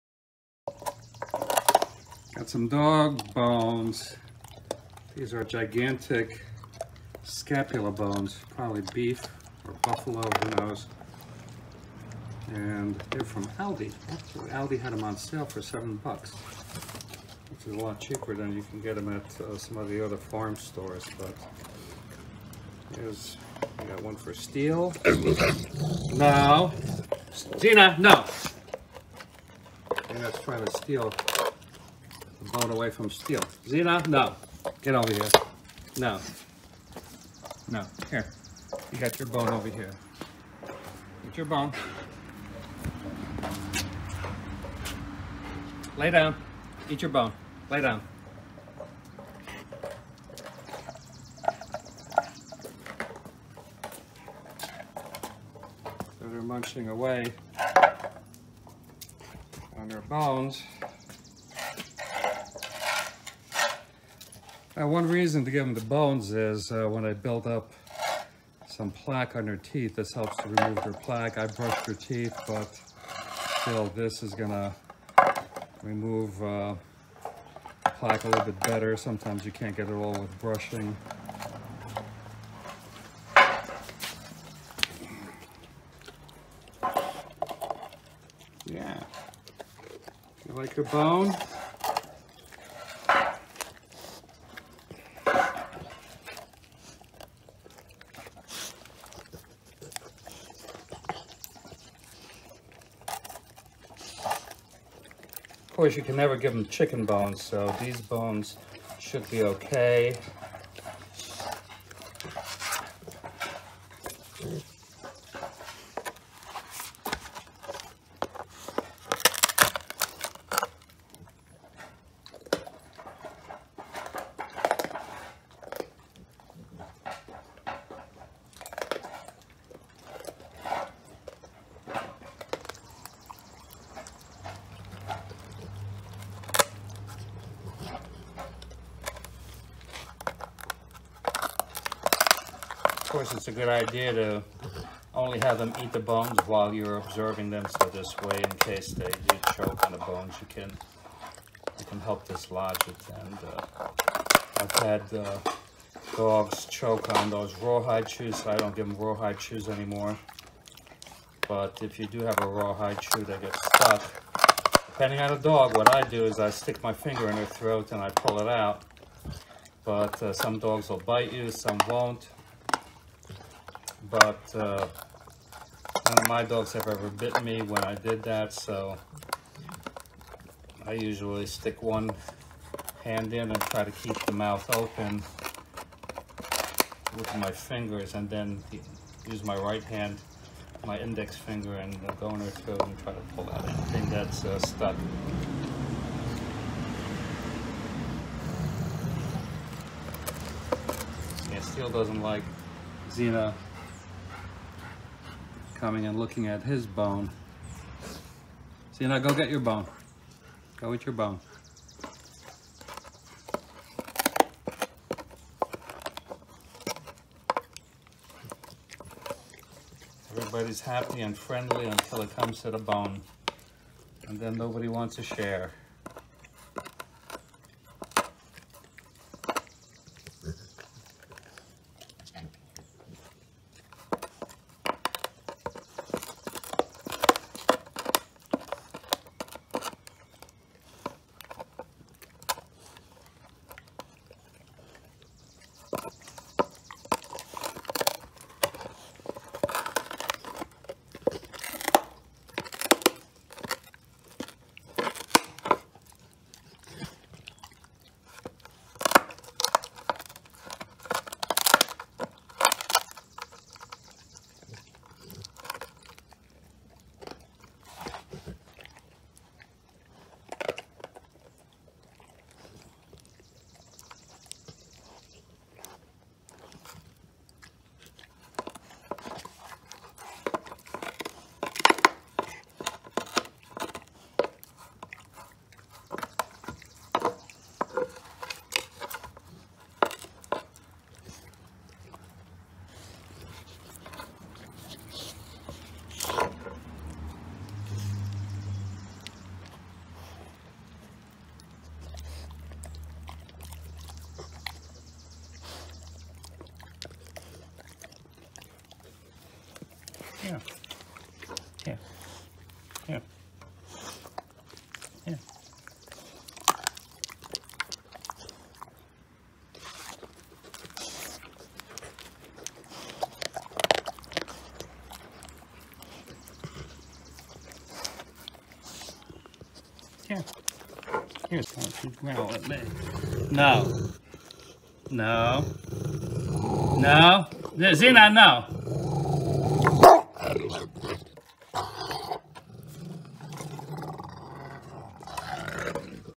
Easiest way to clean your dog's teeth | secrets your vet doesn't want you to know
Do you want to keep your dog's teeth clean?  Or do you want to get ripped off for $400 dollars by your vet?  Yes, that's how much it costs to get your dog's teeth cleaned by your vet.  My vet requires a dental xray and then they have to put your dog to sleep to clean their teeth.  Some dogs have died,  just for a teeth cleaning.  My dogs had some plaque on their teeth.  Yes, I brush them once a week but it still happens.   Instead I bought them some giant scapula bones and let them chew on them for an hour.   It cleaned all the plaque off and their teeth are now sparkling clean.
Why expose your dog to radiation, anesthesia and possible death.

Hope this will help you save money.
Never leave your dog unattended with a bone.